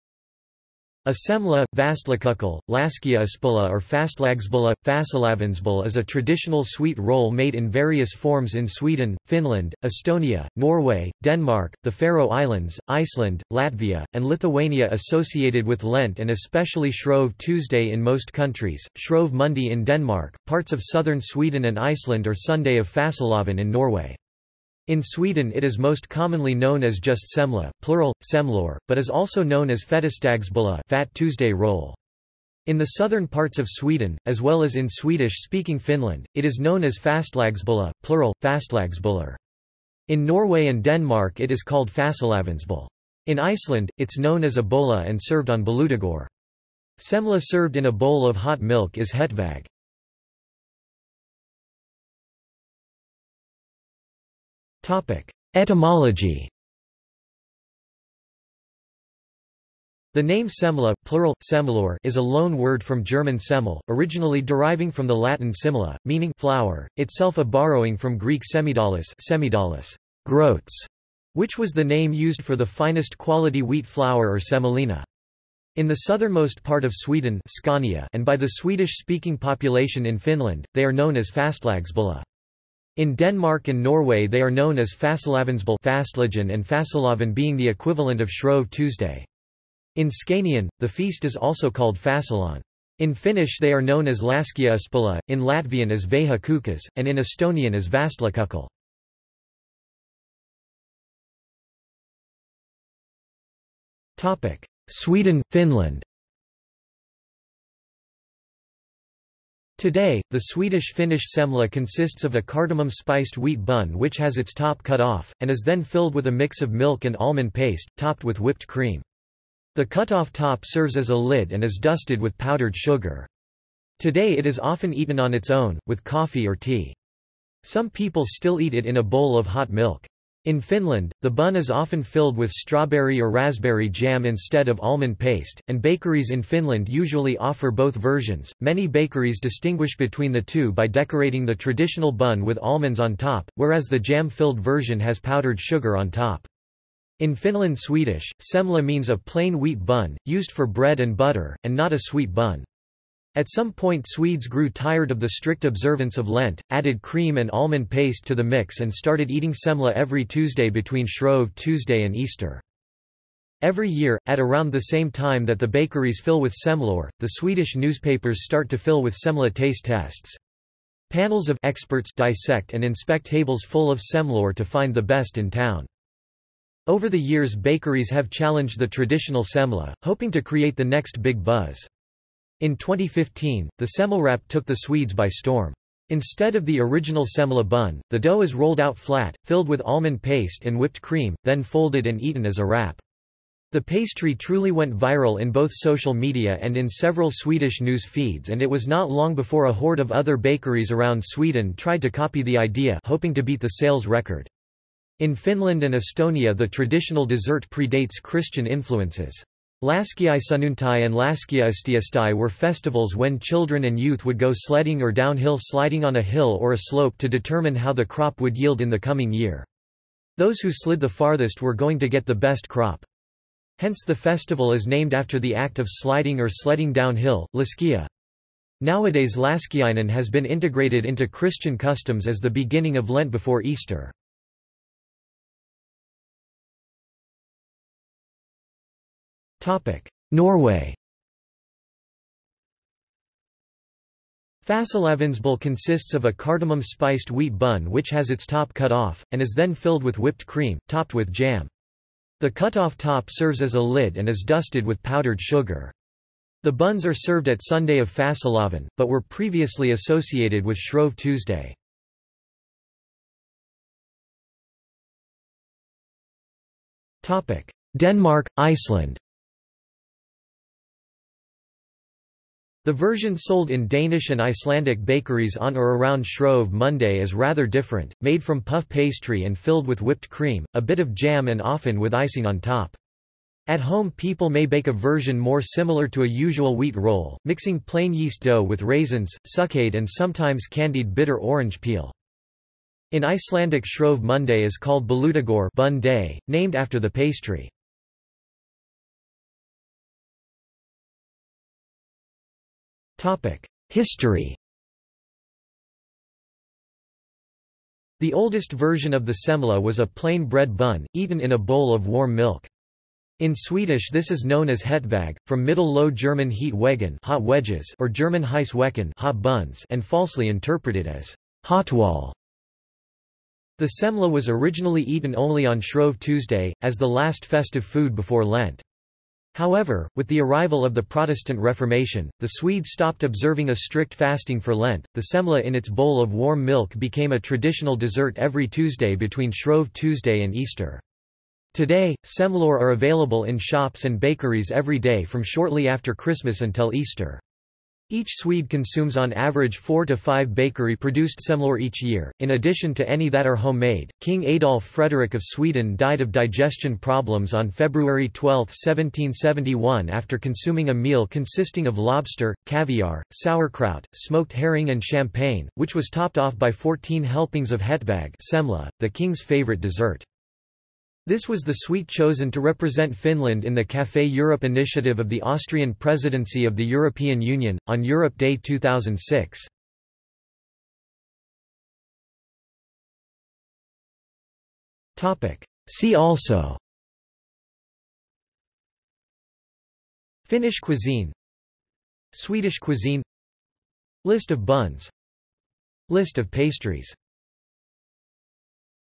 Semla | Wikipedia audio article
This is an audio version of the Wikipedia Article:
Semla

00:00:51 1 Etymology
00:01:40 2 Sweden/Finland
00:03:56 3 Norway
00:04:23 4 Denmark/Iceland
00:04:59 5 History
00:06:21 6 See also

- Socrates

SUMMARY
A   semla, vastlakukkel, laskiaispulla or fastlagsbulle/fastelavnsbolle is a traditional sweet roll made in various forms in Sweden, Finland, Estonia, Norway, Denmark, the Faroe Islands, Iceland, Latvia, and Lithuania associated with Lent and especially Shrove Tuesday in most countries, Shrove Monday in Denmark, parts of southern Sweden and Iceland or Sunday of Fastelavn in Norway. In Sweden it is most commonly known as just semla (plural: semlor), but is also known as fettisdagsbulle (fat tuesday roll). In the southern parts of Sweden, as well as in Swedish-speaking Finland, it is known as fastlagsbulle (plural: fastlagsbullar). In Norway and Denmark it is called fastelavnsbolle. In Iceland, it's known as a bolla and served on Bolludagur. Semla served in a bowl of hot milk is hetvägg.